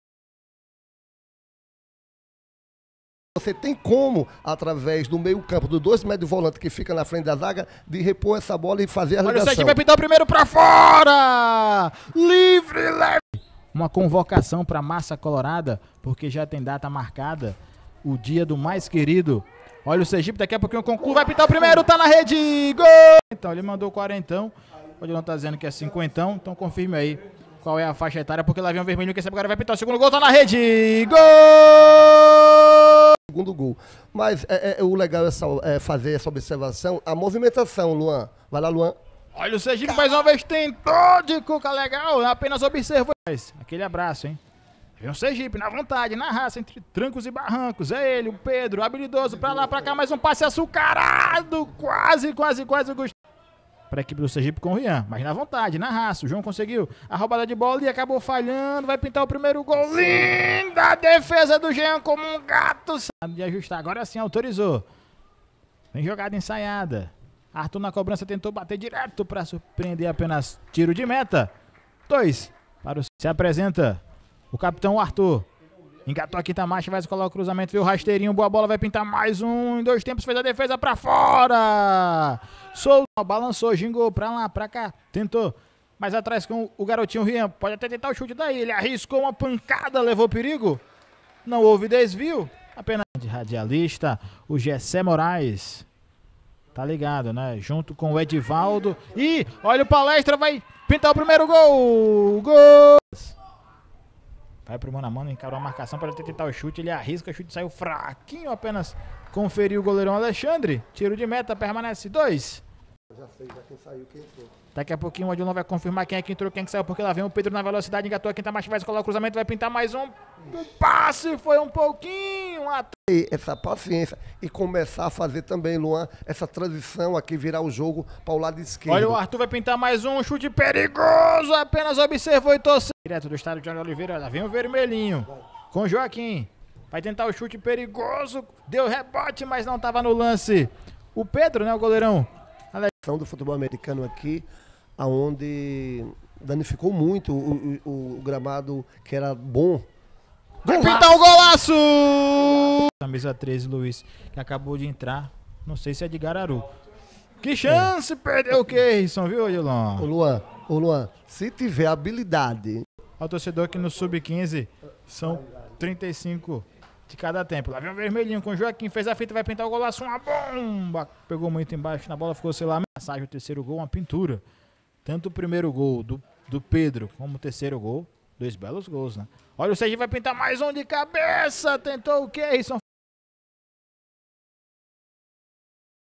MELHORES MOMENTOS - SERGIPE 3 X 1 PALESTRA - CAMPEONATO SERGIPANO - SUB-15 - 2024
Narração: Luan Guarany
Comentarista: Odilon Júnior
Cinegrafista: Alexandre Rocha
Direção: Alexandre Rocha